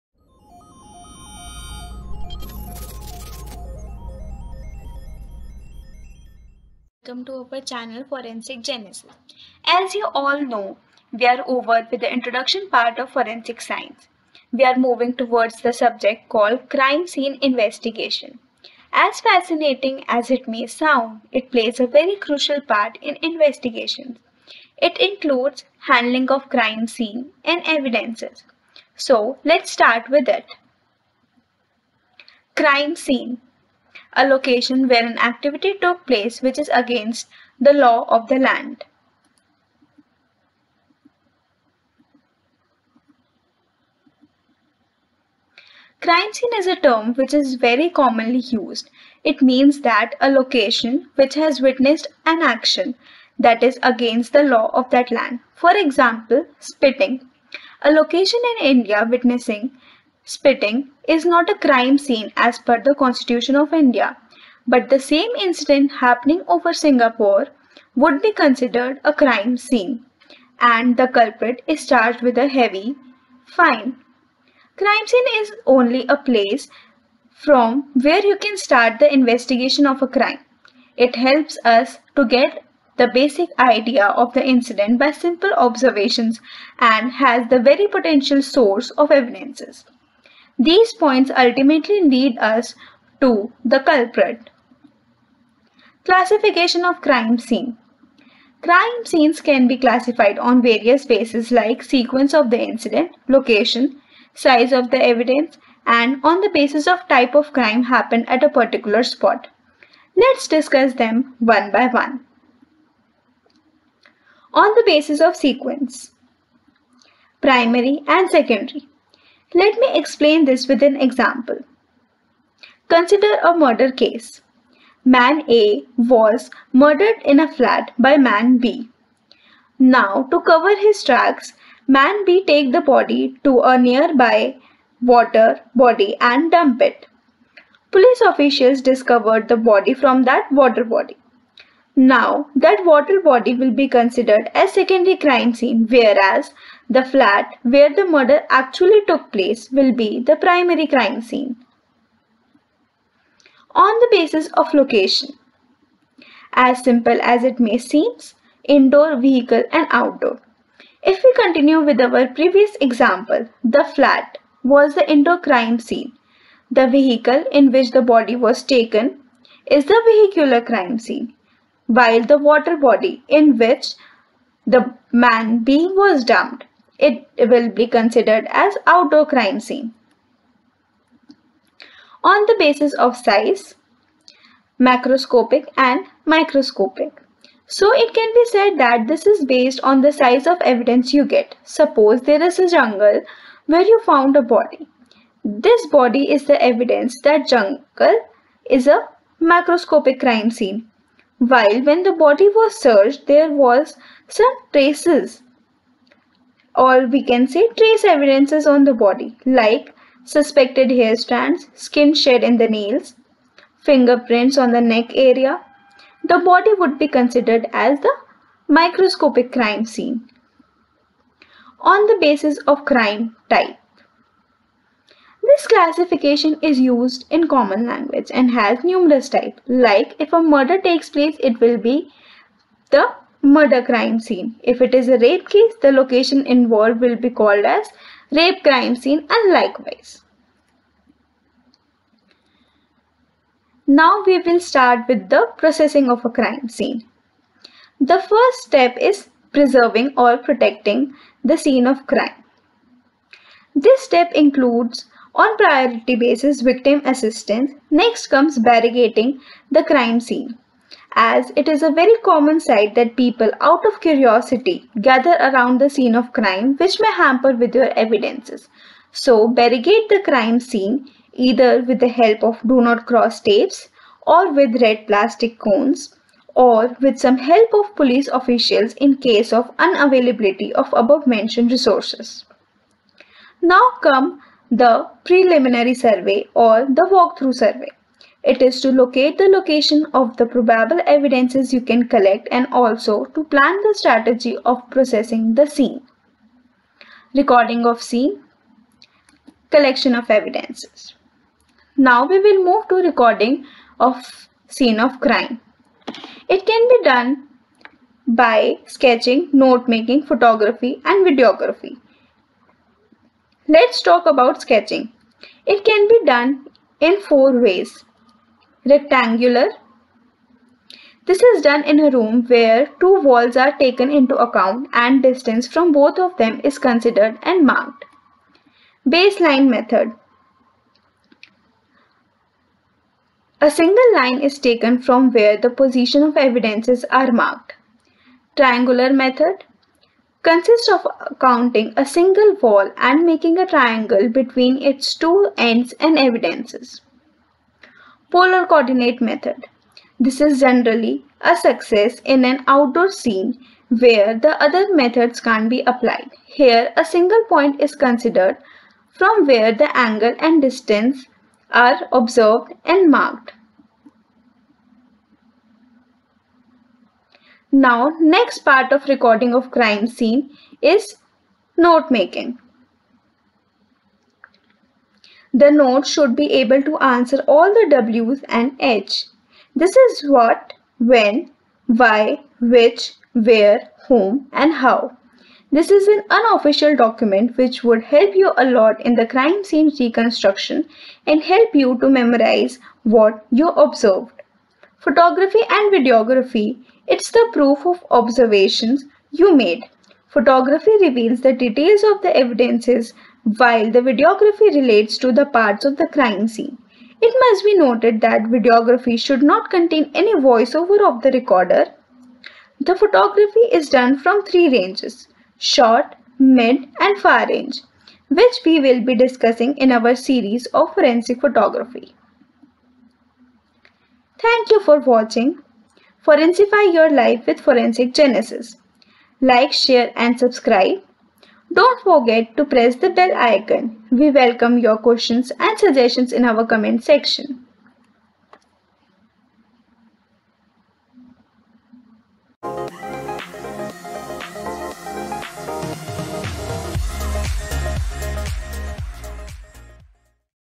CRIME SCENE INVESTIGATION (Lecture-1)
This lecture series will lead you to the aspects and importance of crime scene investigation. Regarding that this lecture is attributed to the definition and classification of the crime scene. It also includes the preliminary processing of the crime scene- a crucial part to nab the culprit.

For any query and suggestions ;